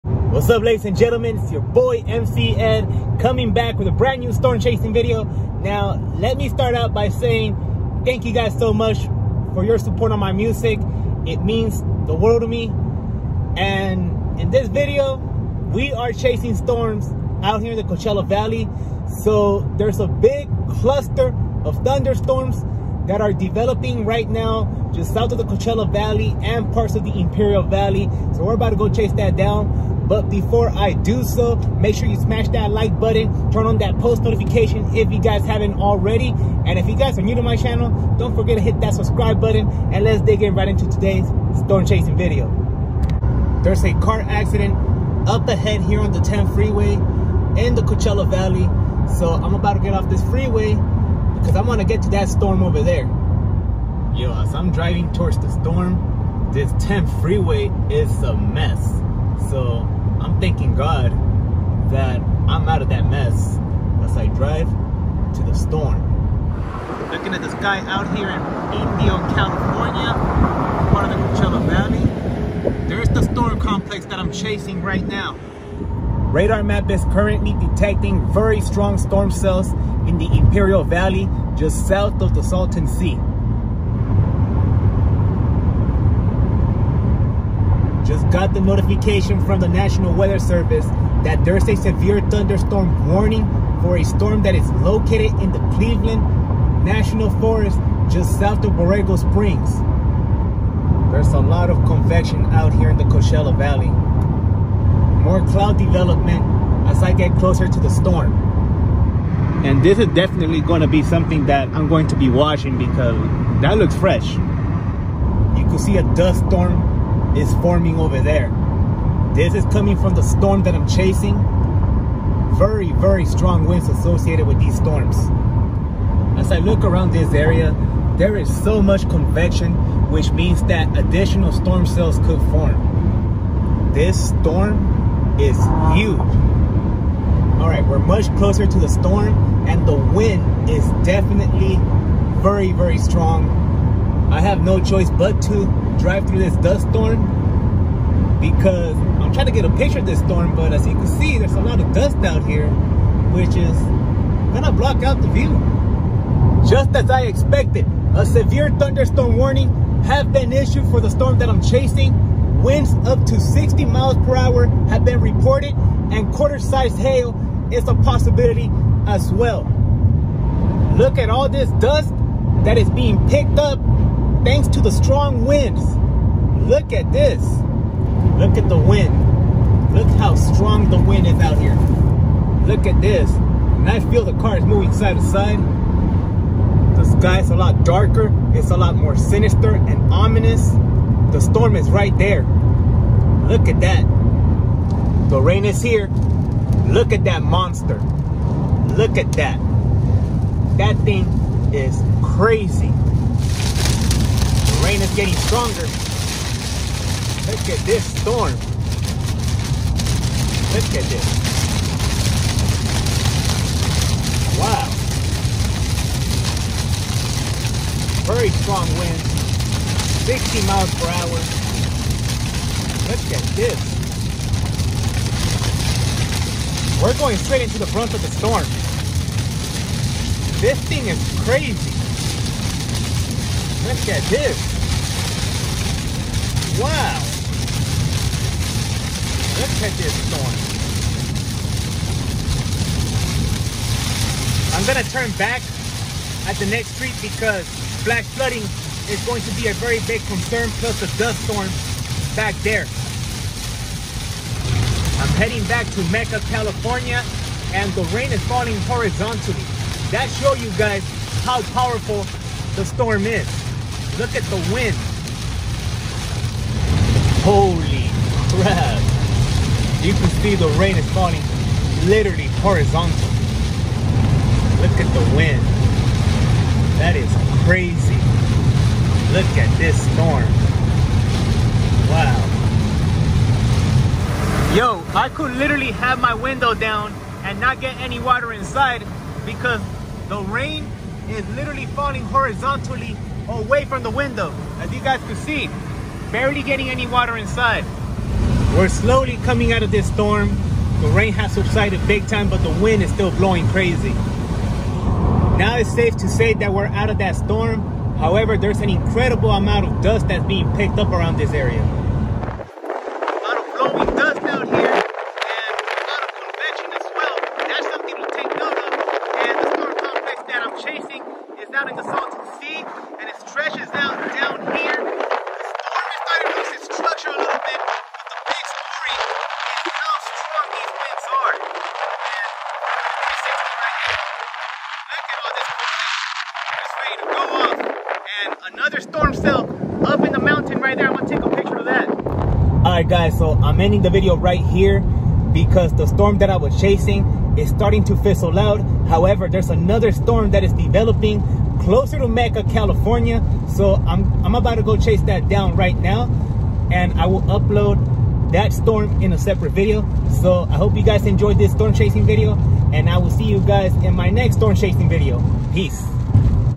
Chasing the Monsoon ⛈️ Severe Thunderstorm with Dust Storm over Salton Sea and Imperial Valley, CA
SEVERE WEATHER OUTBREAK AT IMPERIAL VALLEY IN CALIFORNIA (Westmorland; Brawley, Calipatria, imperial, Calexico, El Centro, Holtville, Niland), SOUTHERN PART OF THE SALTON SEA AND BORREGO SPRINGS

A cluster of strong thunderstorms that originated east of Blythe, CA slowly marched to the Imperial Valley. Additional thunderstorms started developing in the imperial valley as the main cluster of storms started to fizzle out. These storms intensified soon enveloped the Imperial Valley.

Multiple severe thunderstorm warnings were issued for these storms. The main threat from these storms were damaging winds and blowing dust. A dust storm that later covered the Coachella Valley formed here.

Driving conditions became so dangerous that I turned around. The main concern I had driving through this storm was flash flooding.

Additional strong thunderstorms started developing south and west of Mecca, CA while the storm I was chasing weakened below severe levels.

Part two of this storm chasing trip will be uploaded in another video!